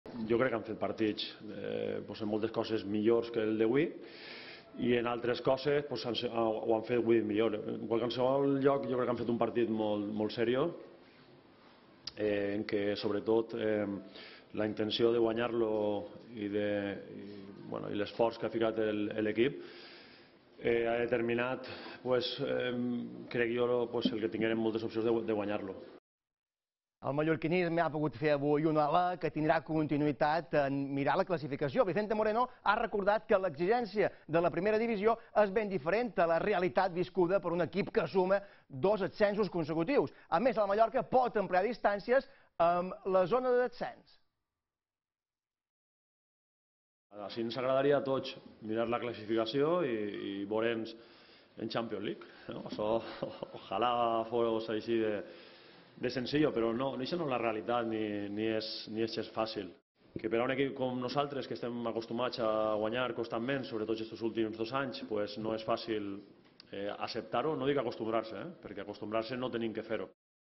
Victòria sense pena
Vicente Moreno ha introduït dos canvis obligats a l’onze inicial. Les absències de Fran Gàmez, per lesió, i Salva Sevilla, per sanció, han donat entrada a Lumor i a Take Kubo. A més, s’ha revelat la incògnita a la porteria. Manolo Reina ha estat l’elegit. El partit s’ha posat de cara ben prest per al Mallorca. Bona jugada individual de Kubo, i Vicente Iborra toma el japonès dins l’àrea. Minuts de tensió per si el VAR corregeix la decisió. Finalment, l’àrbitre assenyala la pena màxima. Lago Júnior agafa la responsabilitat i no falla davant Asenjo. Segon gol del de Costa d’Ivori aquesta temporada i quart partit consecutiu marcant dels mallorquinistes. I passats deu minuts, s’ha repetit la història. Jugada trenada del Mallorca que inicia Kubo amb Dani Rodríguez. Cavalcada del gallec que la passa a Aleix Febas… el català la toca i és envestit pel porter groget. Penal. Aquesta vegada és Dani Rodríguez qui l’agafa i transforma la pena màxima. 2 a 0 i segon gol, també, del de Betanzos a la lliga. Als minuts finals de la primera meitat el Vila-real ha tengut una doble oportunitat per marcar. La primera amb aquest xut de Zambo que atura Manolo Reina i la segona en aquesta rematada de Mario Gaspar que desbarata Lumor.

El segon temps ha començat ben igual que la primera meitat però amb els papers canviats. Aquesta vegada, l’àrbitre ha xiulat penal de Raíllo a Gerard Moreno. Santi Cazorla no ha fallat davant Reina. 2 a 1. Però la reacció del Mallorca no s’ha fet esperar i ha arribat quatre minuts després amb un golàs. Take Kubo envia un míssil amb l’esquerra per batre Asenjo. Amb aquesta diana, el japonès estrena el seu caseller de gols. El segon temps també ens ha deixat el debut de Josep Señé amb la camiseta vermella, que ha sortit al terreny de joc per Kubo que ha estat ovacionat per tot l’estadi. Al final, 3 a UN i victòria del Mallorca tres partits després. Un triomf que manté l’equip de Vicente Moreno fora de les posicions de descens.